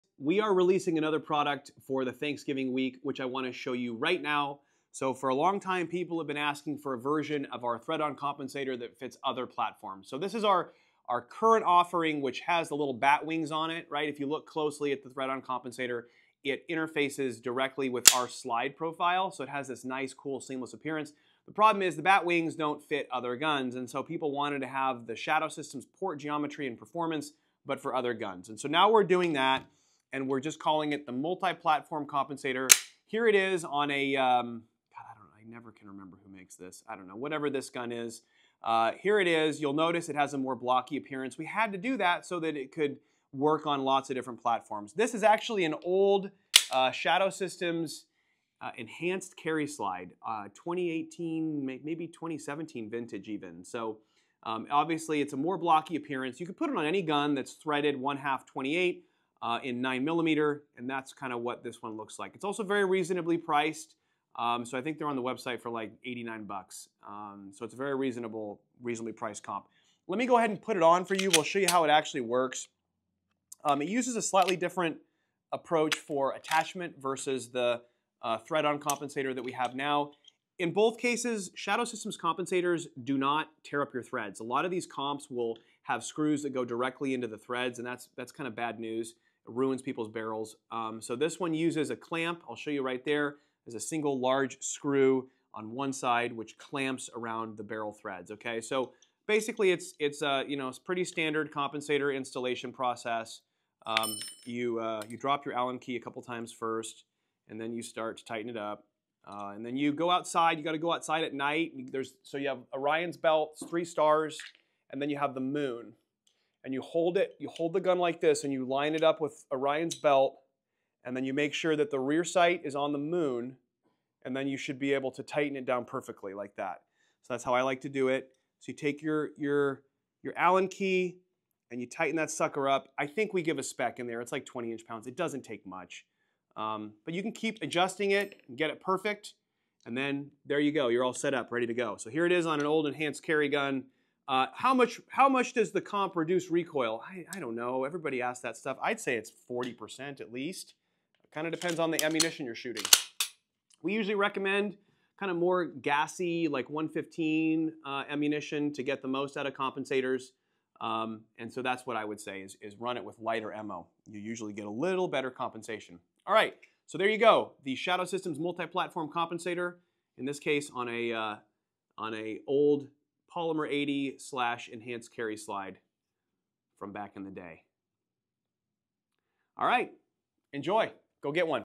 Technical Tuesday: Multi-Platform Comp
The Multi-Platform compensator is designed to fit all ½ x 28 threaded barrel 9mm pistols.  The compensator features highly efficient port geometry and a rock solid mounting system that won’t damage your threads.  This video details installation of the comp and how to make use of the moon when aligning the assembly on your slide.  Good luck!